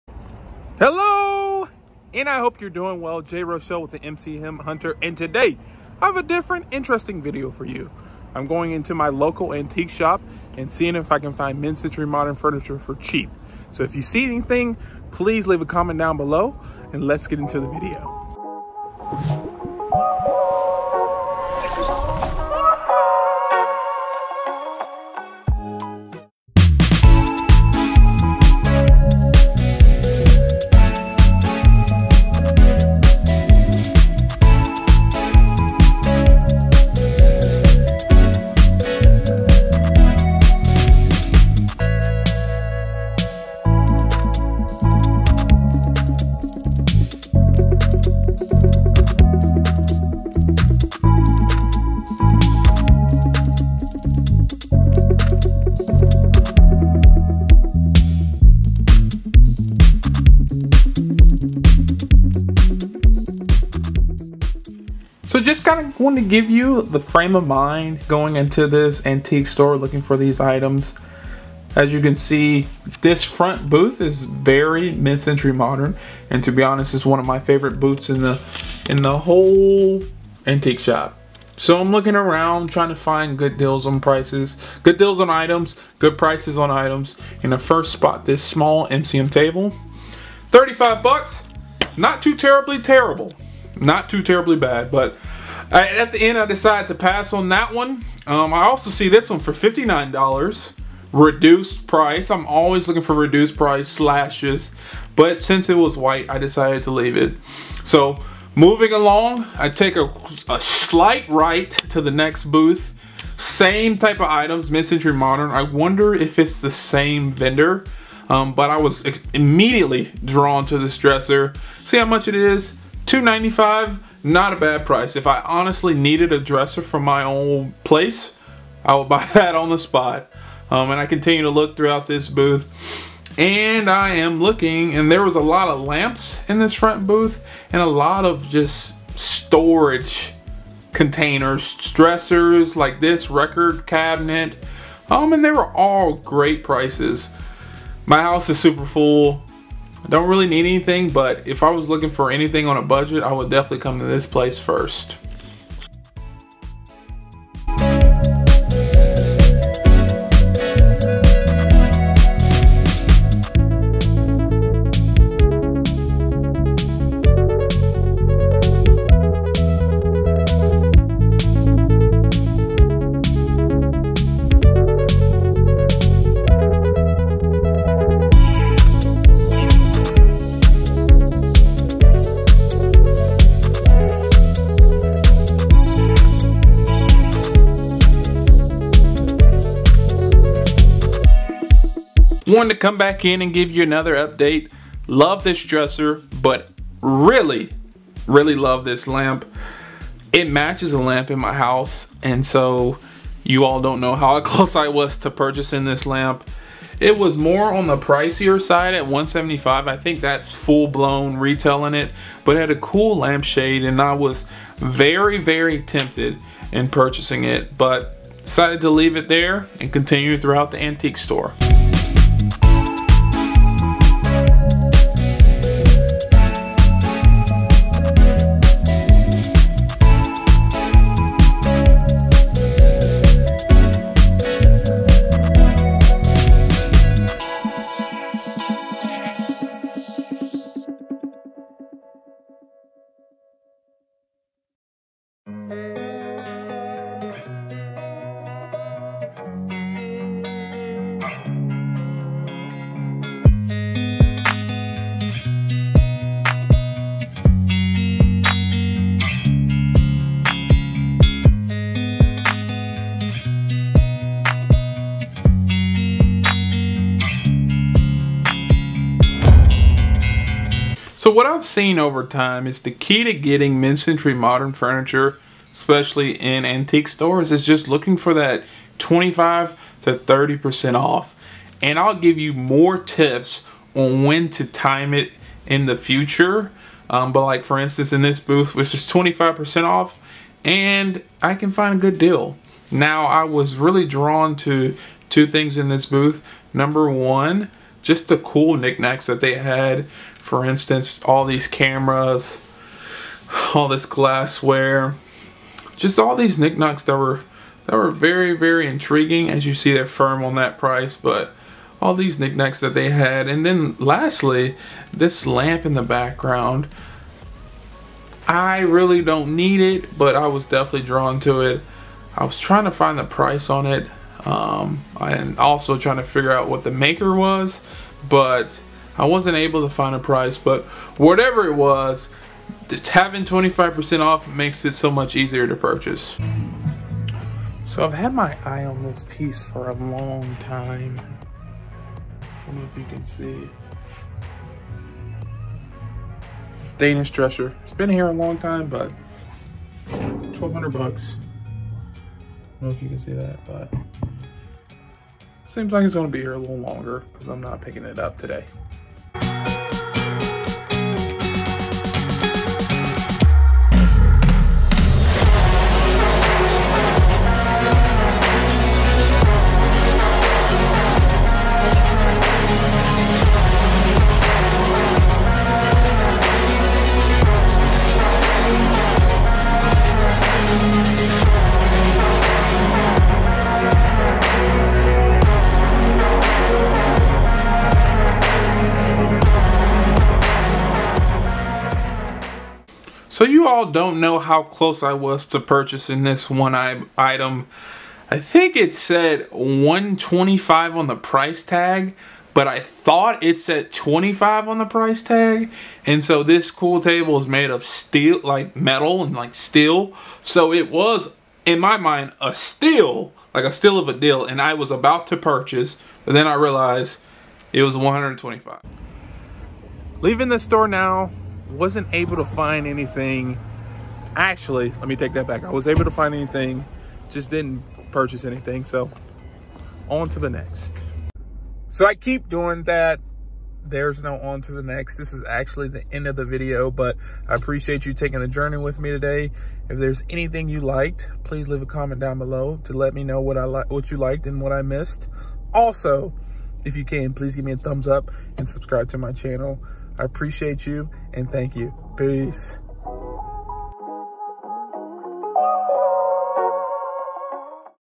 Finding CHEAP Mid Century Modern and Vintage Furniture at Antique Store
Never fear! The MCM Hunter is here! It's never truly about the destination but really about the journey.

Today is vintage and MCM creators markets. Tomorrow, who knows!